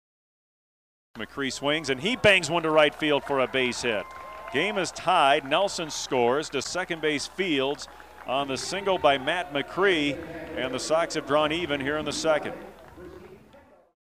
Matt Macri RBI single drives in Chris Nelson 4-13-2011
Matt Macri drives in Chris Nelson to tie the score in the 2nd inning on Wednesday April 13th, 2011.  The Sox would eventually fall to the Reno Aces 7-4.